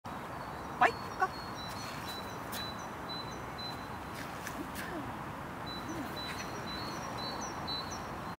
Paikalla istuminen ohjaajan liikkuessa koiran taakse
Koiran tulisi istua paikallaan liikuttamatta jalkojaan, vaikka ohjaaja liikkuisi koiran taakse, sivulle tai muuhun suuntaan.